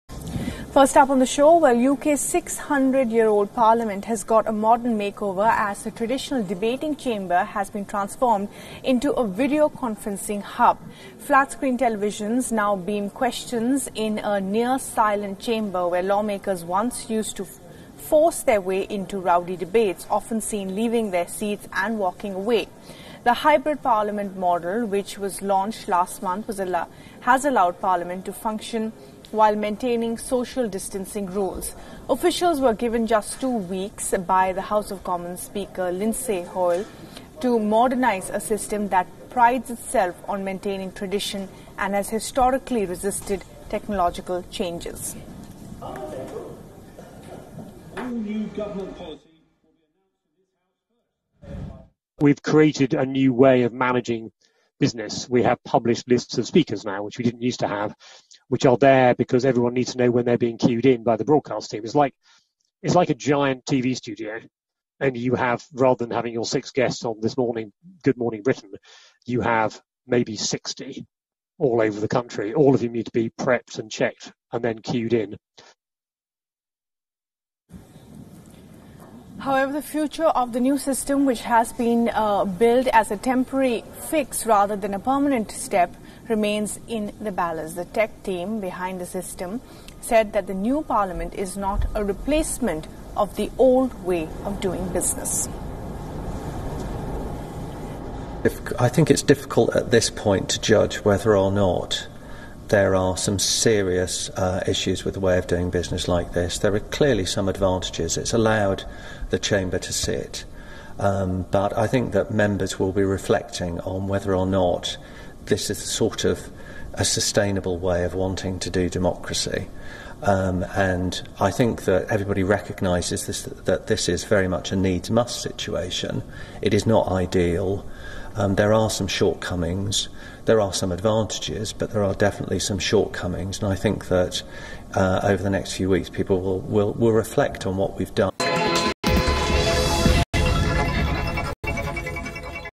UK's 600-year-old parliament's digital makeover | World News | WION News
After 600 years of painstakingly slow evolution, Britain's parliament took a leap into the future when the coronavirus pandemic struck, transforming its deeply traditional debating chamber into a modern videoconferencing hub.

WION: shorturl.at/fwKO0
Zee News English: shorturl.at/aJVY3
Zee News Hindi: shorturl.at/eorM1
Zee Business: shorturl.at/hpqX6
DNA News: shorturl.at/rBOY6
BGR: shorturl.at/eioqL